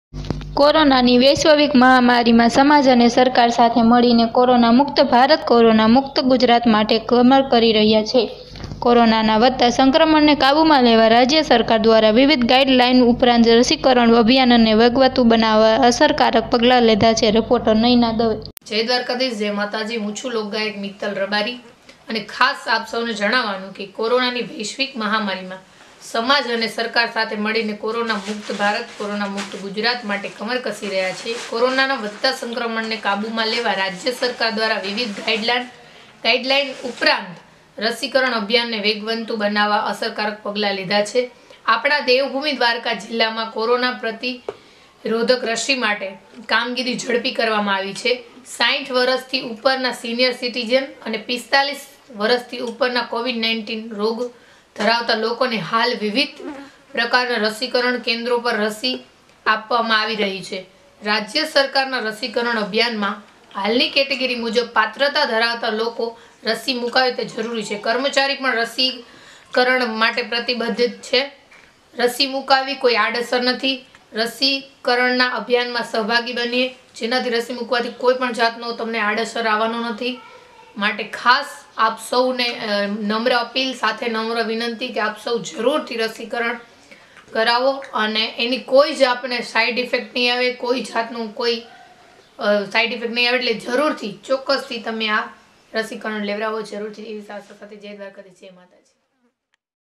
લોકગાયીકા મિત્તલબેન રબારી ની અપીલ
કોરોનામુક્ત ભારત- ગુજરાત માટે રસીકરણ અભિયાનમાં સહભાગી થઇએ-લોકગાયીકા મિત્તલબેન રબારી ની અપીલ

જામનગર

રિપોર્ટર નયના દવે

કોરોનાની વૈશ્વિક મહામારીમાં સમાજ અને સરકાર સાથે મળીને કોરોના મુક્ત ભારત-કોરોના મુક્ત ગુજરાત માટે કમર કસી રહ્યા છે. કોરોનાના વધતા સંક્રમણને કાબુમાં લેવા રાજ્ય સરકાર દ્વારા વિવિધ ગાઇડલાઇન ઉપરાંત રસીકરણ અભિયાનને વેગવંતુ બનાવવા અસરકારક પગલા લીધા છે. ત્યારે દેવભૂમિ દ્વારકા જિલ્લાના લોકગાયીકાશ્રી મિત્તલબેન રબારીનો માહિતી ખાતાએ સંપર્ક કરતા તેમણે એક વિડીયો દ્વારા જણાવ્યું હતું કે, કોવિડની રસી સુરક્ષિત છે. વૈજ્ઞાનિકોના અથાક  પરિશ્રમ થકી રસી આપણા સુધી પહોંચી છે. આપણા દેવભૂમી દ્વારકા જિલ્લામાં કોરોના પ્રતિરોધક રસી માટે કામગીરી ઝડપી કરવામાં આવી છે. ૬૦ વર્ષથી ઉપરના સિનિયર સીટીજન અને ૪૫ વર્ષથી ઉપરના કો-મોરબીડ રોગ ધરાવતા લોકોને હાલ દેવભૂમિ દ્વારકા જિલ્લાના વિવિધ રસીકરણ કેન્દ્રો પર રસી આપવામાં આવી રહી છે. લોકગાયીકાશ્રી મિત્તલબેન રબારીએ રસીકરણ અંગે વધુમાં કહ્યું હતું કે કોરોના સામેની સંજીવની ઔષધિ તરીકેનો એક માત્ર શ્રેષ્ઠ ઉપાય કોરોના વિરોધી રસી જ છે, ત્યારે નાગરિકોએ સરકારી દવાખાનામાં વિનામૂલ્યે મળતી અને ખાનગી હોસ્પિટલોમાં નજીવા દરે ઉપલબ્ધ રસી મુકાવી જ લેવી જોઈએ. રાજ્ય સરકારના રસીકરણ અભિયાનમાં હાલની કેટેગરી મુજબ પાત્રતા ધરાવતા સૌ  લોકો રસી મુકાવે તે જરૂરી છે. તેઓએ કહ્યું કે કર્મચારી પણ રસીકરણ માટે પ્રતિબધ્ધ છે. રસી મૂકાવાથી કોઇ આડઅસર થતી નથી માટે કોરોના મુકત ભારત-ગુજરાતમાં રસીકરણના અભિયાનમાં સહભાગી થઇએ તેમ તેઓએ અનુરોધ કર્યો હતો.